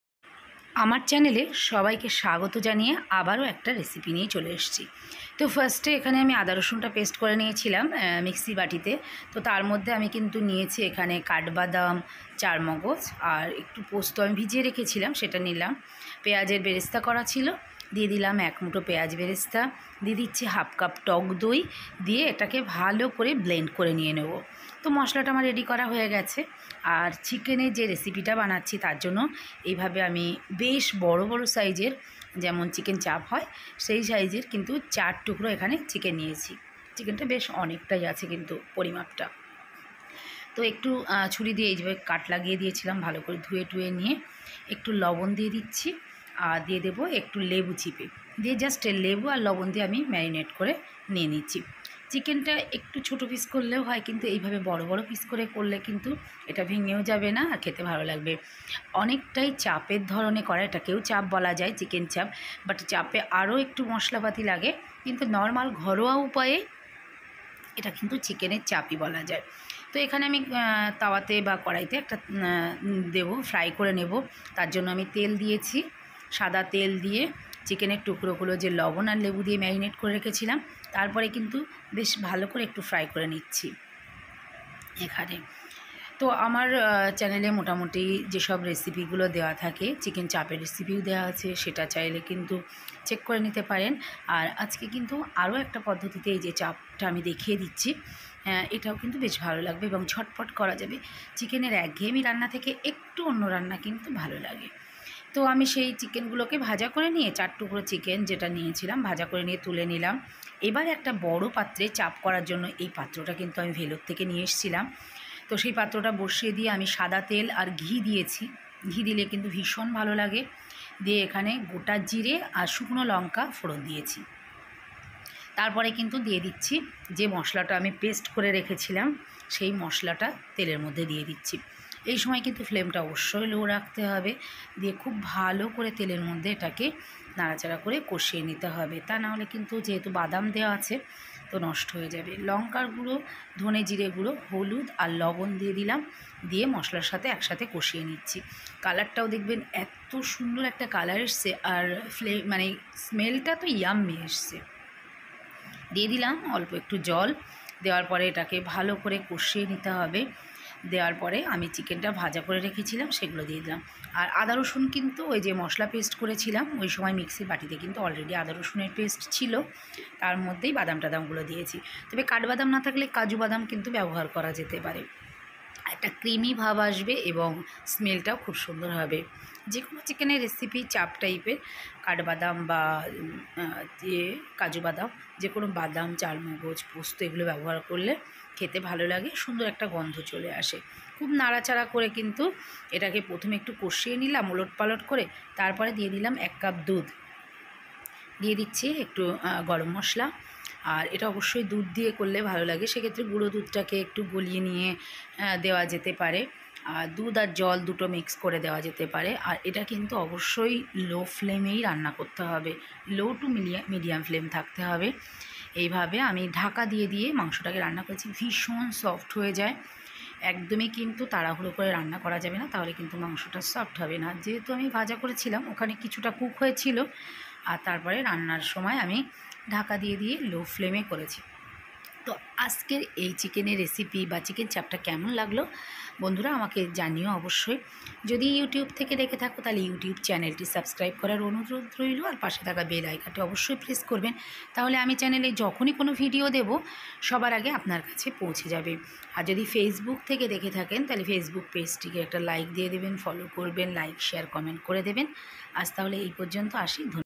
জাস্ট অপূর্ব স্বাদের chicken chap ,😋😋😋😋 একদম সহজ পদ্ধতিতে বাড়িতে বানিয়ে নিন 😋😋❣️❤️
Shelly kitchen is a unique channel.Easy way to make special dishes desserts. Follow us in fb , x handle,instagram